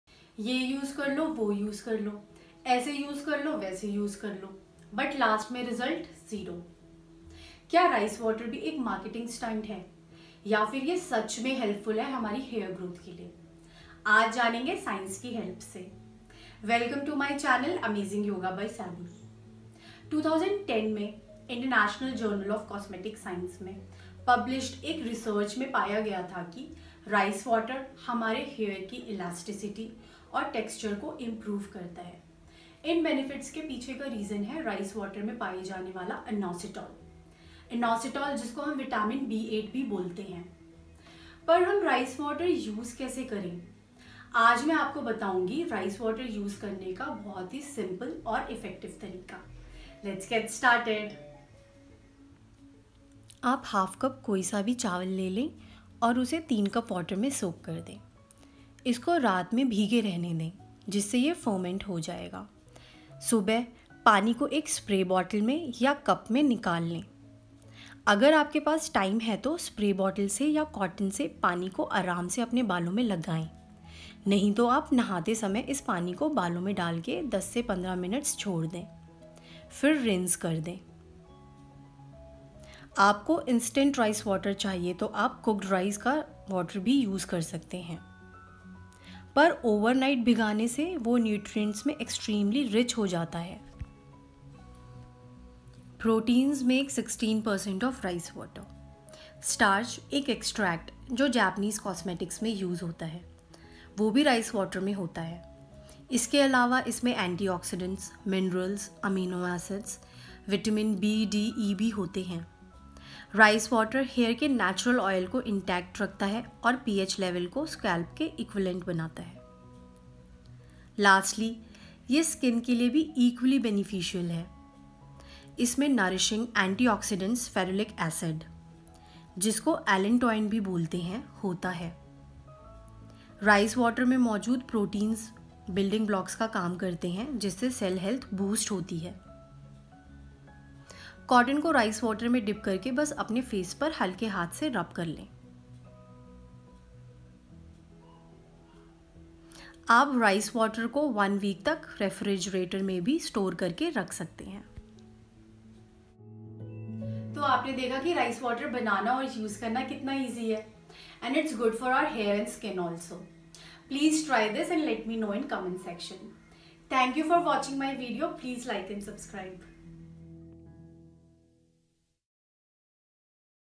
Rice Water for Hair and Skin | Good or Bad | What Science Says | S-Vyasa YIC Cert.
Does rice water really work? Is there any scientific logic behind the efficacy of rice water for hair and skin? If yes, how to prepare rice water and how to use rice water? Let's find answers to all questions. Proteins make 16 percent of rice water. Starch, an extract used in Japanese cosmetics, is also present. Besides these, rice water is rich in antioxidants, amino acids, minerals and Vitamins B, D, E. International Journal of Cosmetic Science says that a research found that rice water improves hair elasticity and increases texture. The reason behind this is presence of Inositol in rice water. Inositol is also known as Vitamin B8.
Researchers found that rice water keeps natural oil of hair intact and makes the pH level equivalent to scalp. Rice water also contains a nourishing antioxidant, ferulic acid, also called allantoin, which acts as building block to boost cell health. You can hence use rice water on skin, especially face, as well. The easiest and most effective way to use rice water is to leave half cup rice in 3 cups water overnight to let it ferment. In the morning, strain the water and apply it either using a spray bottle or you can just pour it in your hair and leave for 15 minutes before washing off.